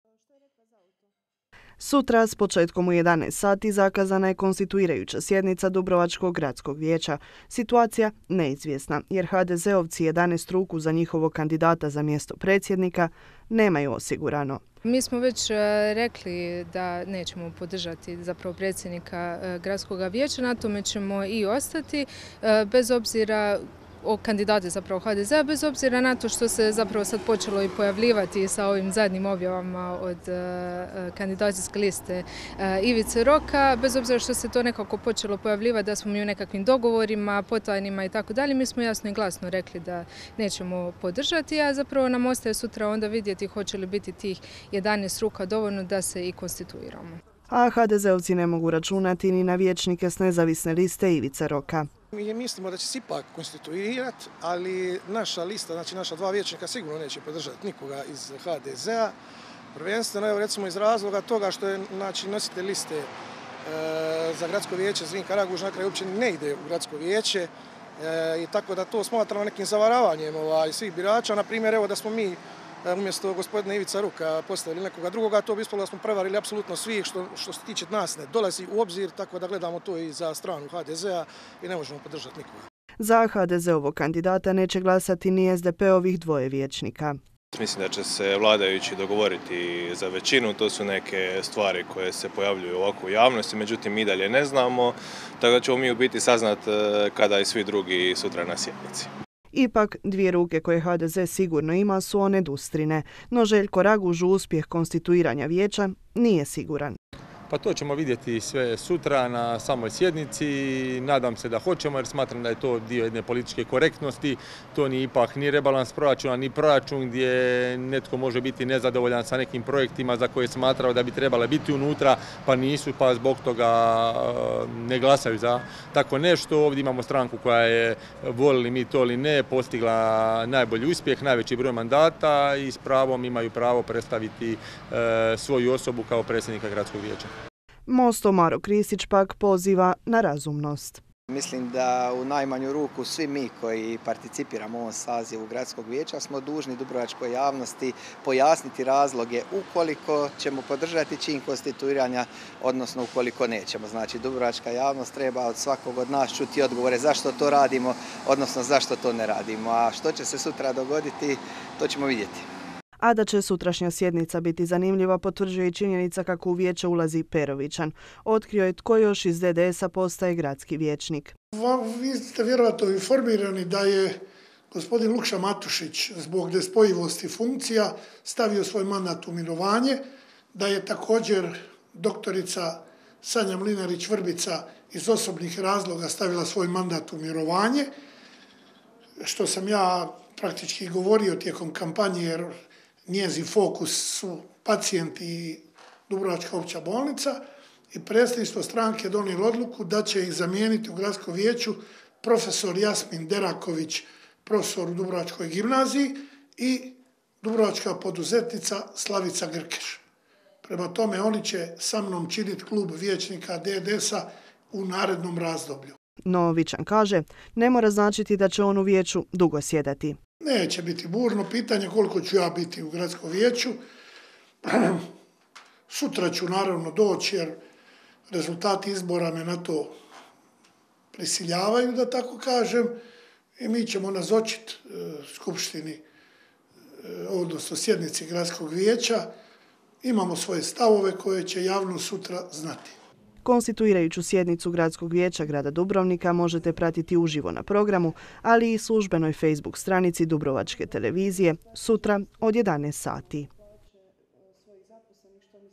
Hoće li se sutra konstituirati dubrovačko Gradsko vijeće?
Na sutrašnjoj konstituirajućoj sjednici Gradskog vijeća bit će sigurno iznenađenja-poručuju to gradski vijećnici s kojima smo danas razgovarali. Hoće li HDZ imati većinu, još nije poznato, no njihovog kandidata za predsjednika Gradskog vijeća sigurno neće podržati ni Srđevci, ni SDP ni Nezavisna lista Ivice Roka.